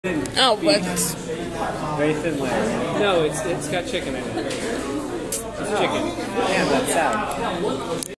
Lemon Chicken 2009-12-4
Have a sweet tooth, but want chicken? You could get this from the Sun Hong Kong Restaurant on Durant, just west of Telegraph in Berkeley, but that place is now closed.